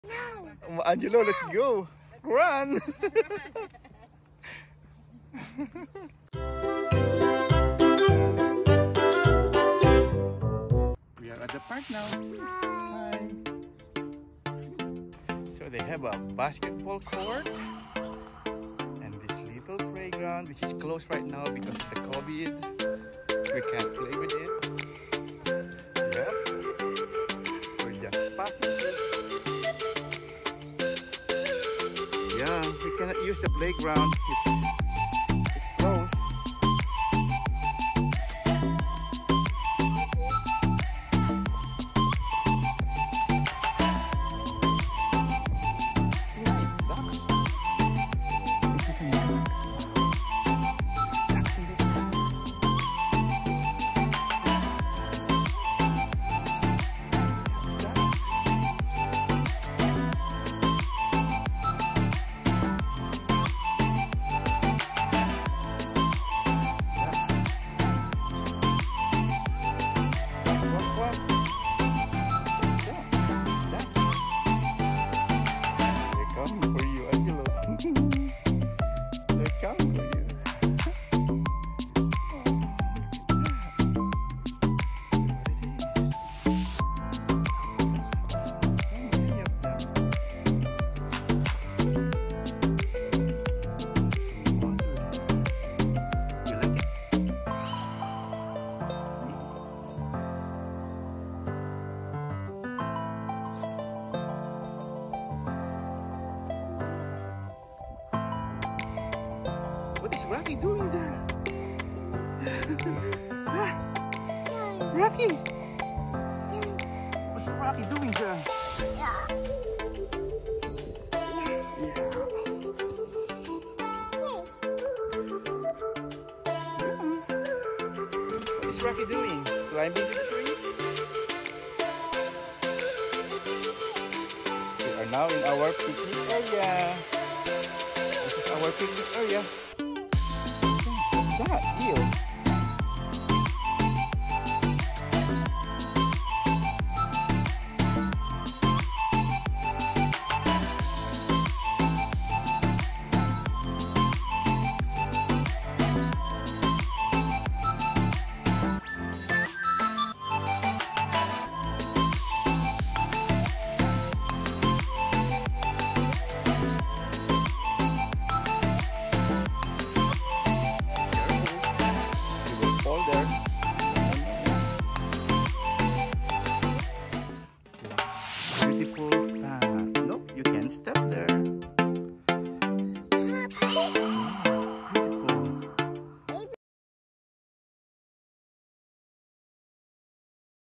Marita Payne Pond 👉 Angelo Play at Marita Payne Pond | David Lewis Park
Angelo Play at Marita Payne Pond located at David Lewis Park.